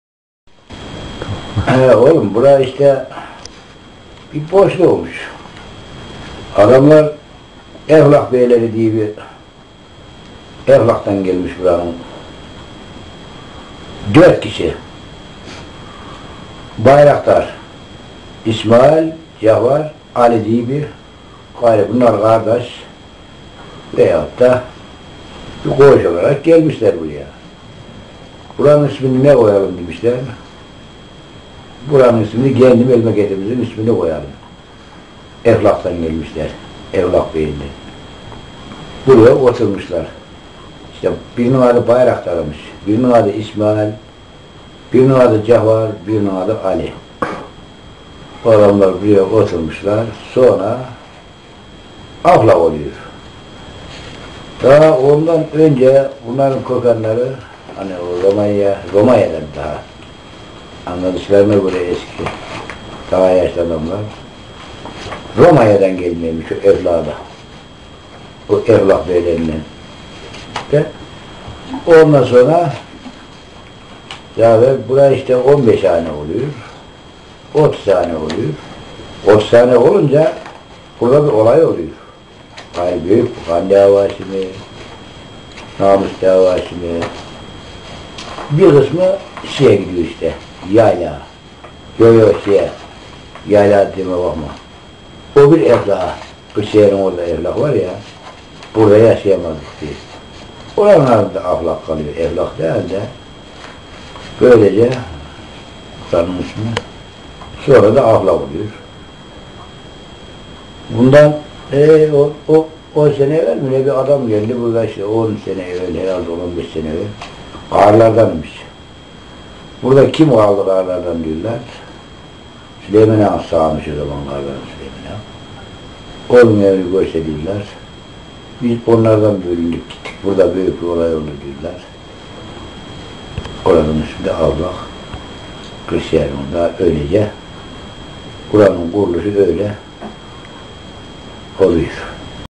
NURİ BAYRAKLI AFLAK KÖYÜNÜN TARİHİNİ ANLATIYOR
NURİ BAYRAKLI